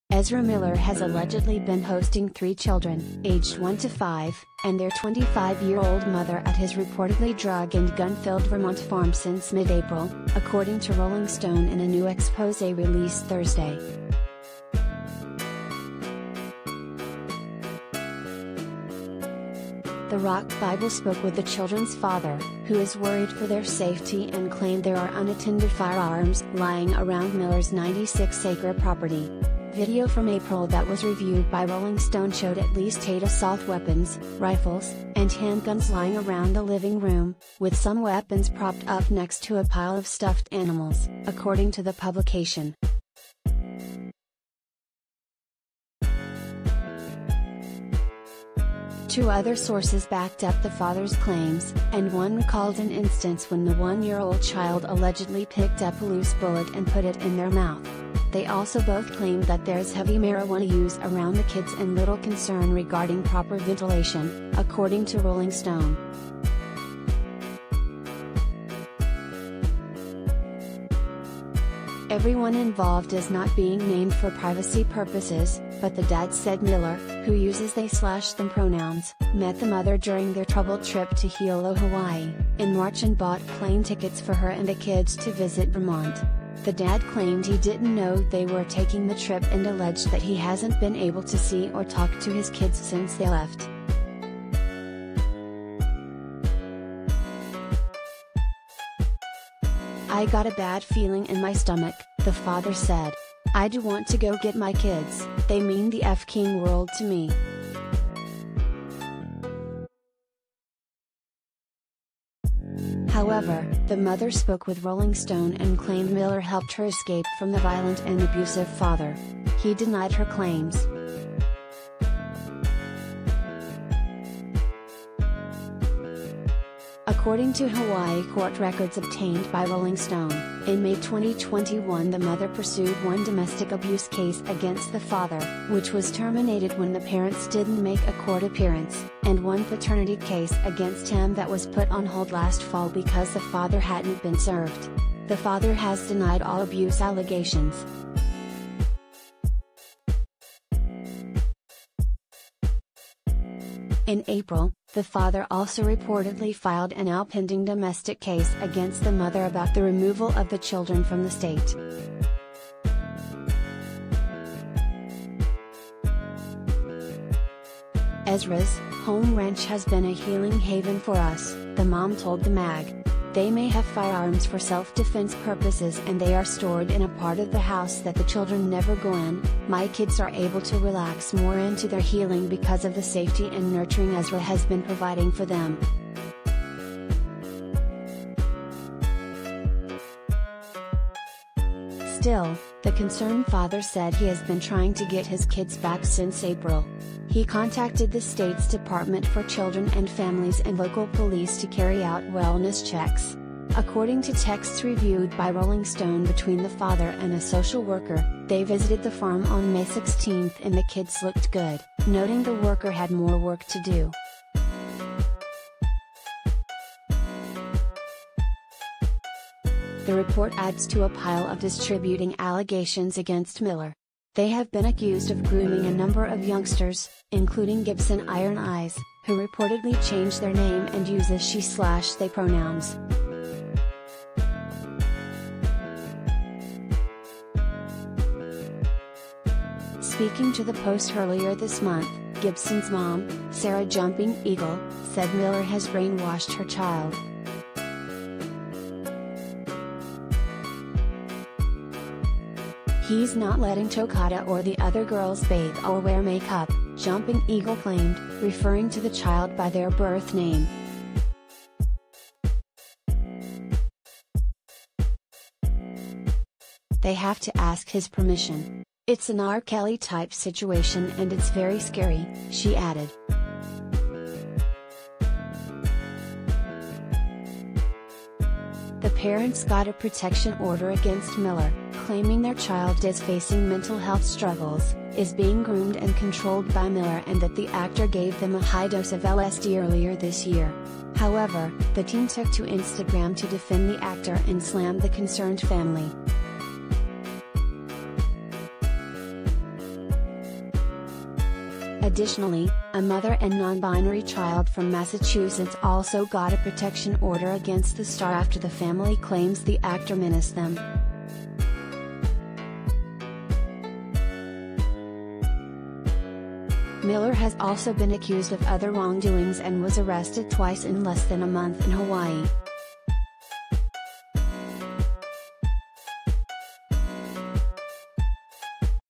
Ezra Miller housing 3 kids, their mom at gun-filled Vermont farm: report
Ezra Miller has allegedly been hosting three children, aged 1 to 5, and their 25-year-old mother at his reportedly drug- and gun-filled Vermont farm since mid-April, according to Rolling Stone in a new exposé released Thursday.
The rock bible spoke with the children’s father, who is worried for their safety and claimed there are unattended firearms lying around Miller’s 96-acre property. Video from April that was reviewed by Rolling Stone showed “at least eight assault weapons, rifles, and han